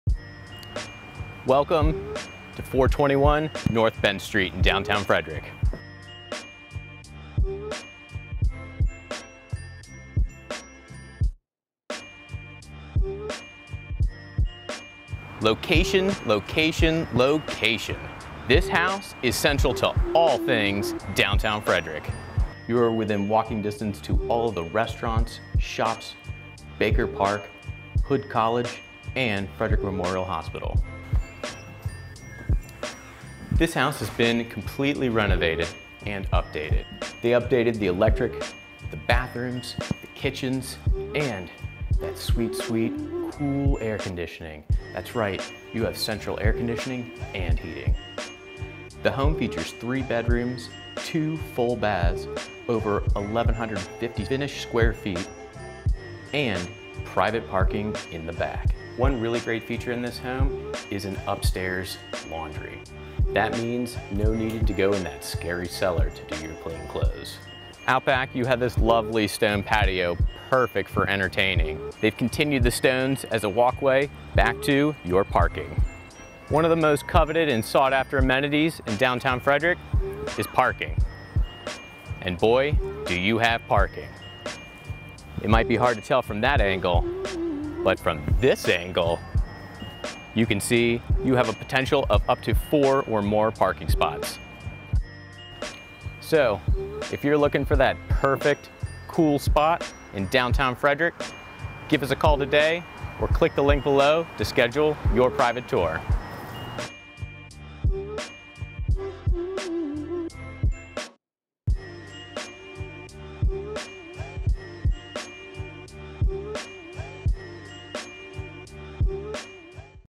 Renovated Downtown Victorian
Check out this adorably Renovated Downtown Victorian!
421 N Bentz St. Frederick, MD
3 Bed | 2 Bath | 1,152 Sq Ft
Beautifully renovated home in downtown, with off-street parking! Hardwood floors throughout. Laundry on bedroom level - or can convert back to bedroom. Large back yard with custom stone patio in back. Close to Hood, Baker Park, Hospital, and Downtown Restaurants & Shopping.